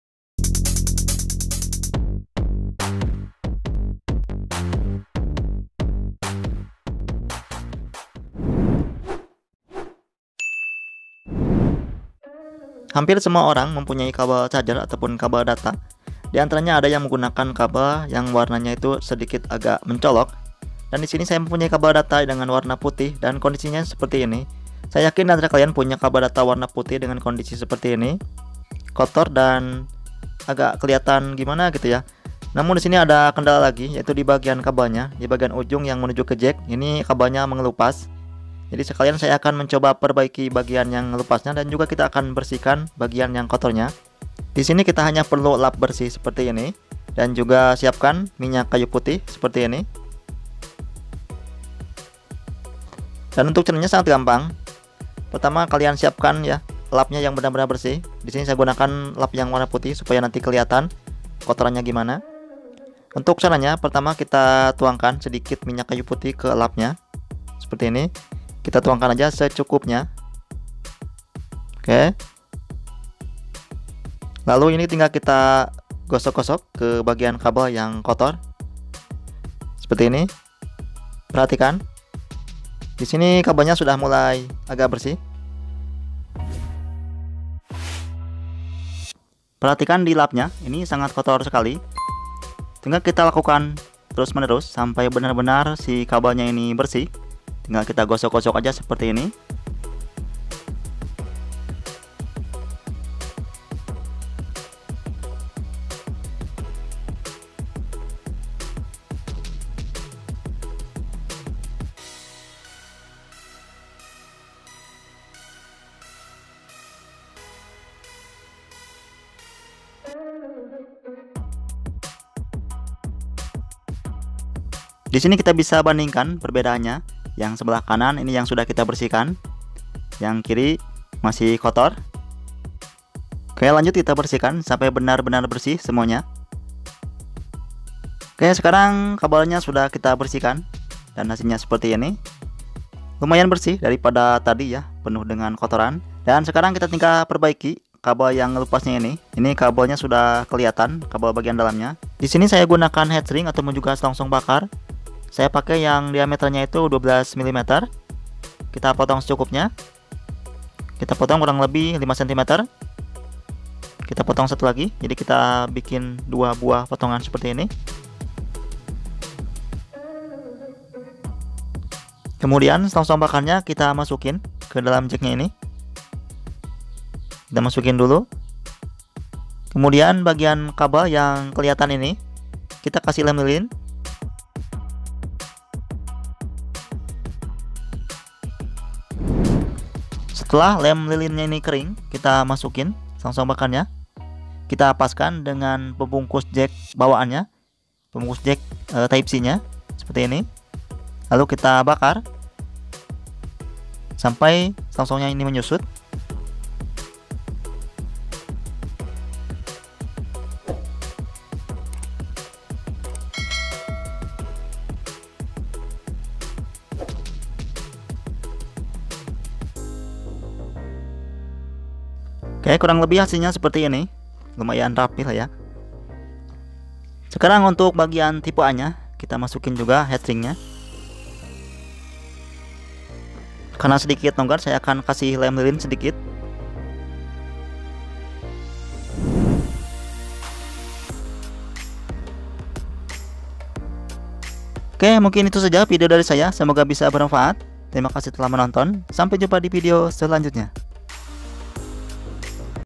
Cara Membersihkan Kabel Charger Hp Yang Kotor - Service Kabel Data
membersihkan kabel data terutama kabel yg warna hitam itu pasti kotor ada noda hitam dikabelnya divideo ini kita akan bersihkan sekaligus memperbaiki kabel yang rusak atau robek ,simak videonya sampai selesai.